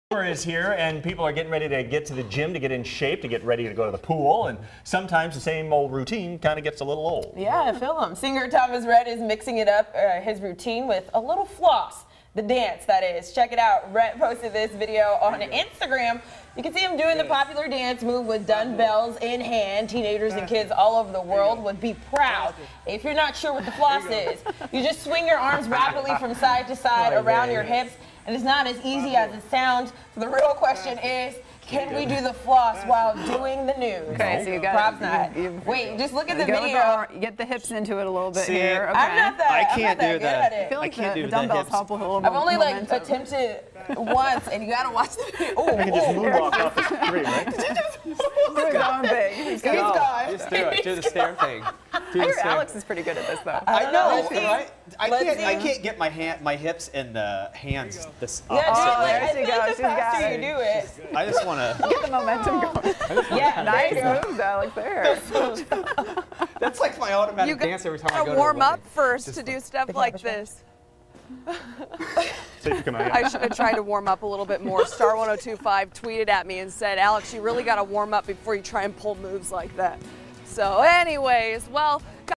KCCI anchors attempt 'The Floss'
Eric Hanson, Rheya Spigner, Elizabeth Klinge, Riley O'Connor and Alyx Sacks attempt to the dance.